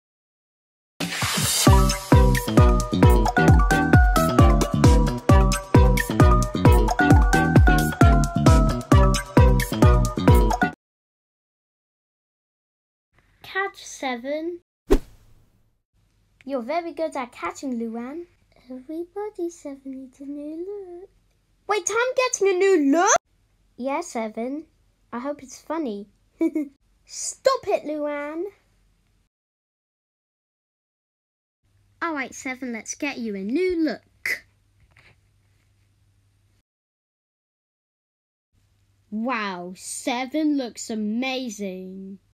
The Luan Show | Seven’s New Look | Ep.2
The Luan show presents Odo numberblock 7 Luigi Isabelle Luan and Steve

Seven is getting his new look and the others will like it

The Luan Show Is For All Ages